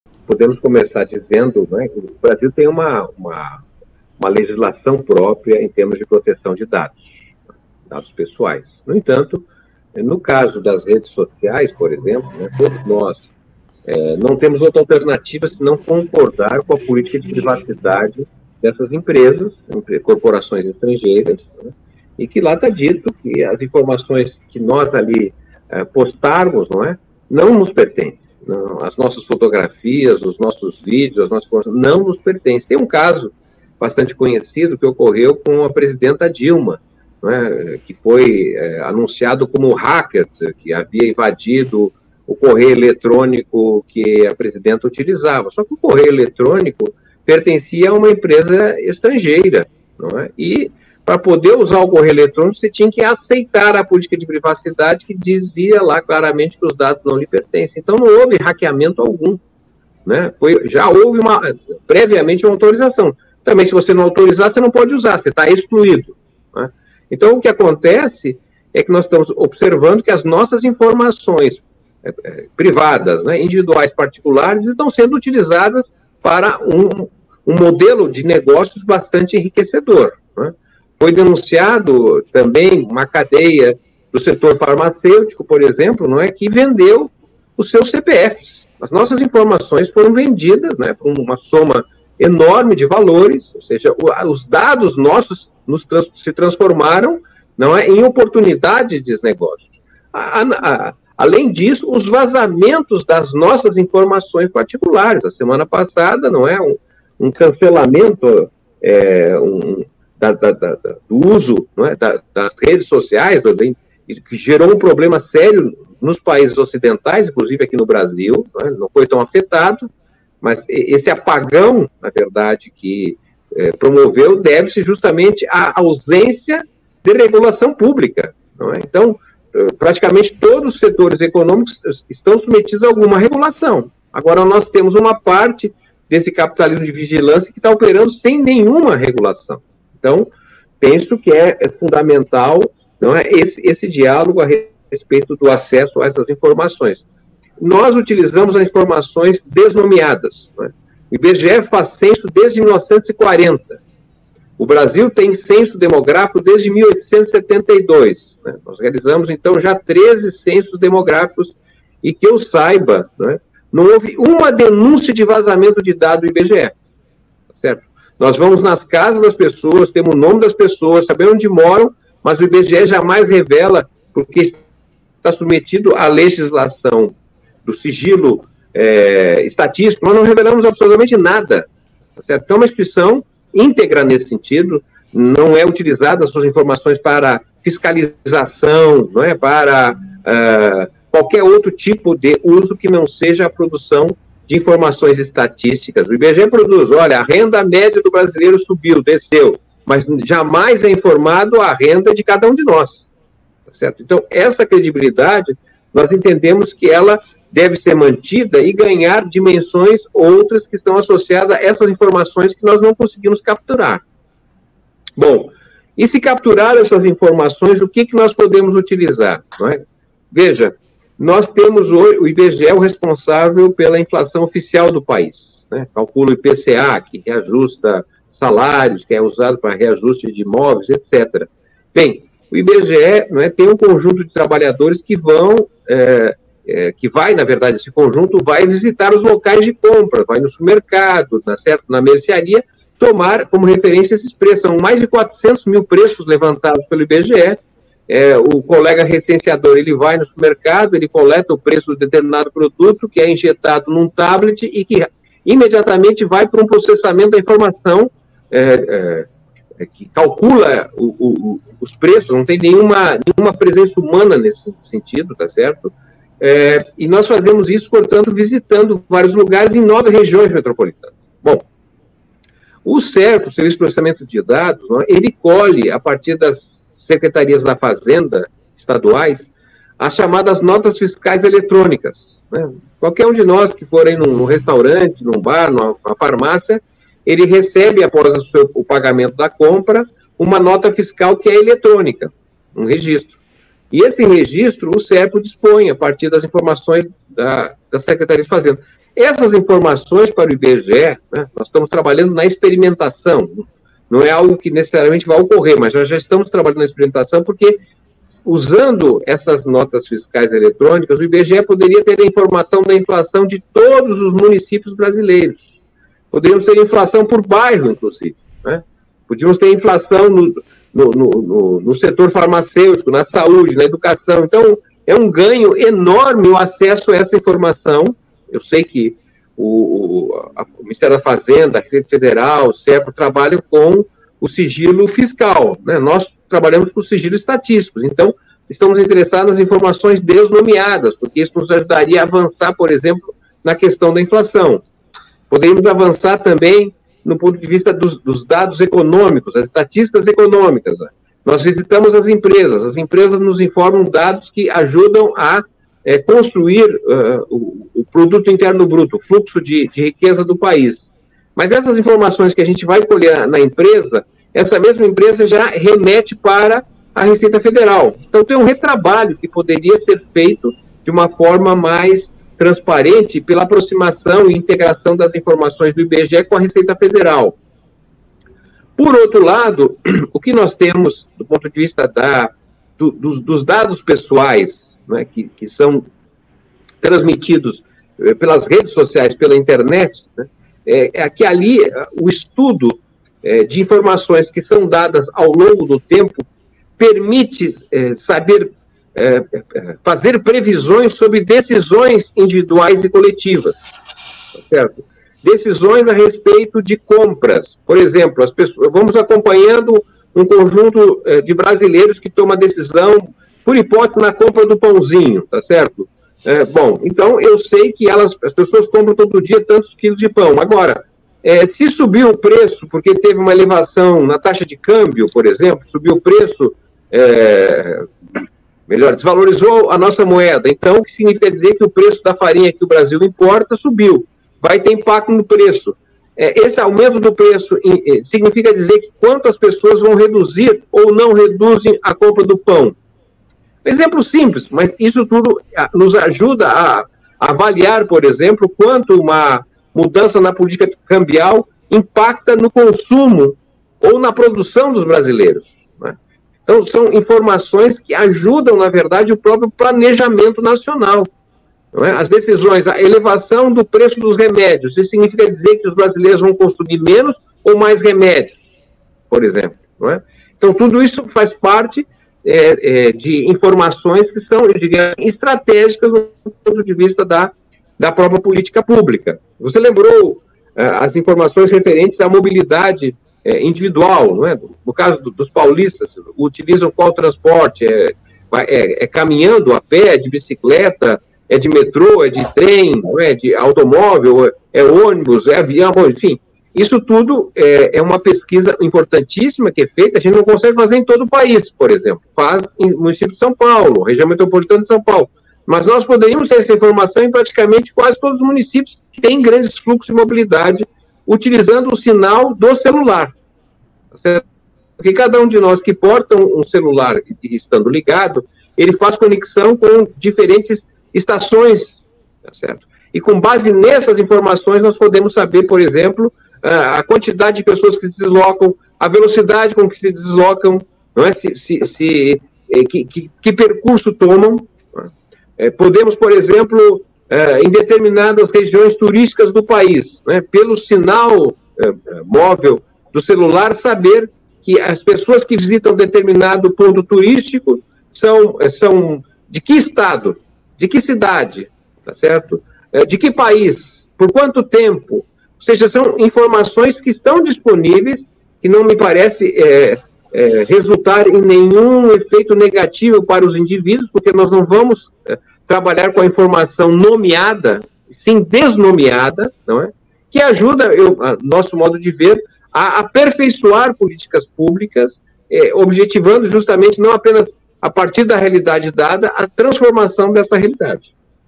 Segurança, privacidade e o uso de dados para aprimorar políticas públicas
O IBGE usa informações "desnomeadas", o que garante a privacidade de dados, diferentemente das gigantes de tecnologia que tem acesso a dados dos brasileiros. "Que eu saiba, não houve uma denúncia de vazamento de dados do IBGE", aponta o presidente do Instituto Brasileiro de Geografia e Estatística (IBGE), Marcio Pochmann, fazendo uma analogia com diversos escândalos que já envolveram as big techs.

OUTRAS PALAVRAS